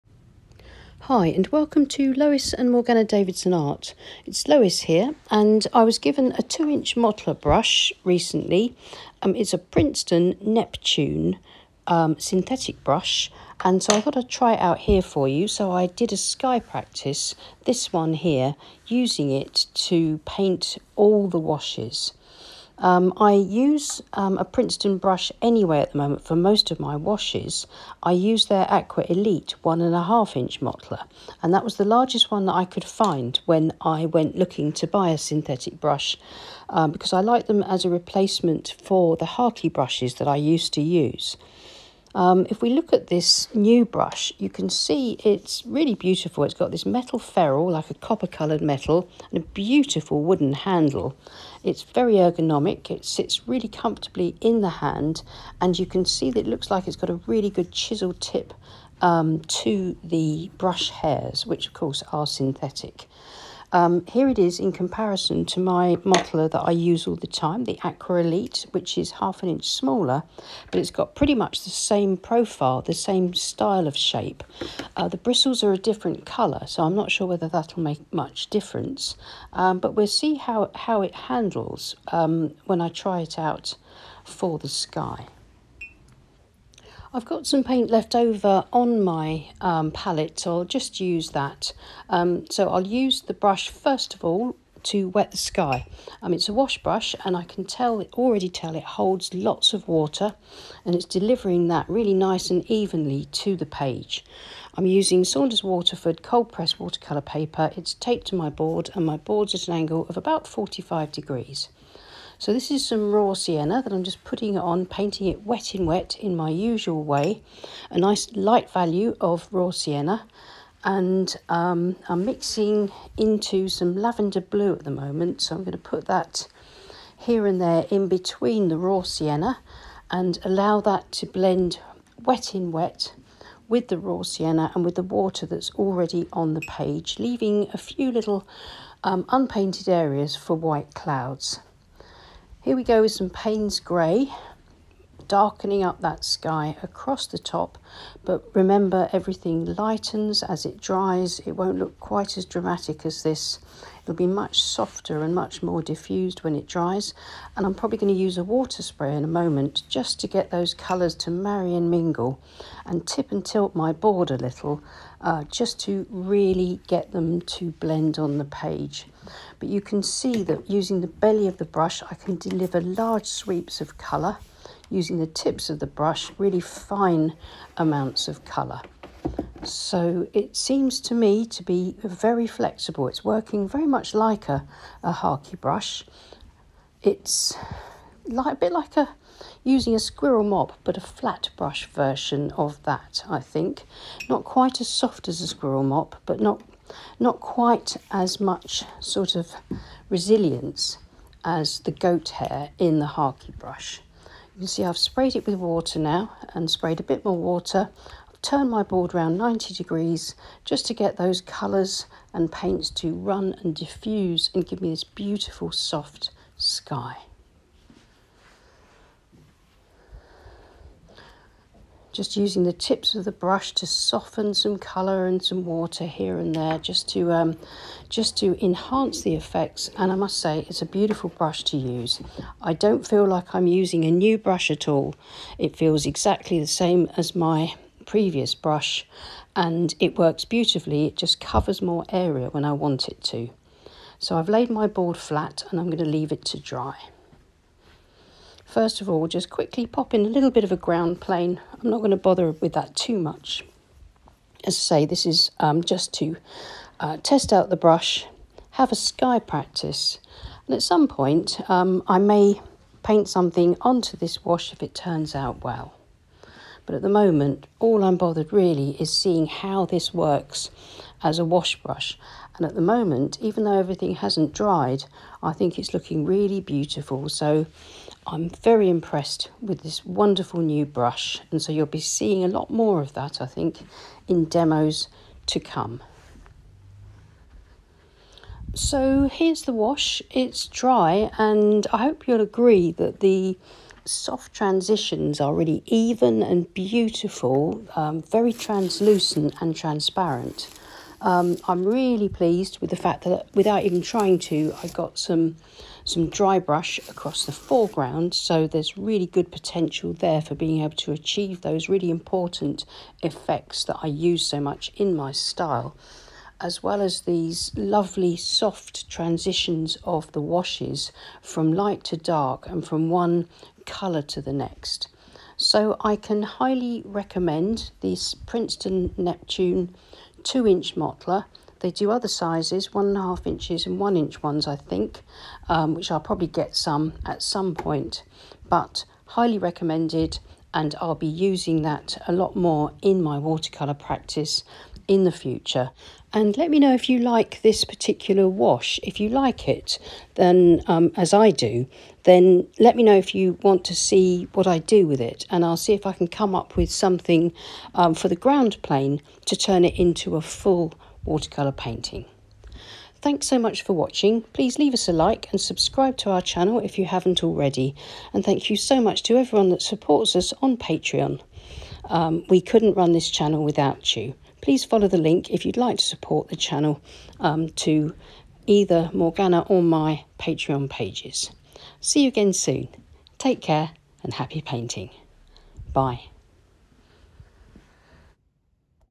IS THIS BRUSH BETTER THAN A HAKE? Beginners Watercolor Painting Landscape SKY Tutorial BRUSH REVIEW
Hi! It's Lois here, trying out and reviewing the PRINCETON NEPTUNE 2" MOTTLER BRUSH - it's a synthetic big brush, but is it better than a HAKE brush?
This brush was a gift from Morgaine, and the video is not sponsored in any way, so a completely independent review

Purchase Morgaine's original paintings